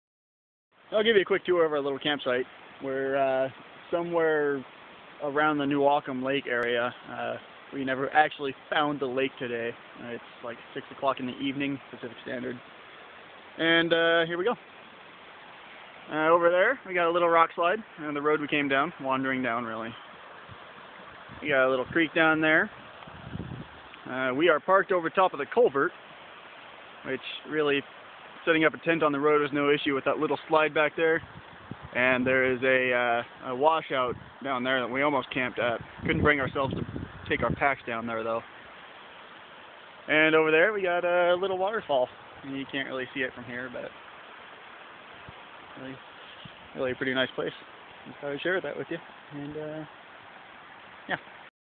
Camp Tour.3gp
A brief tour of our camp on the failed Newaukum Lake Expedition. June 3, 2011.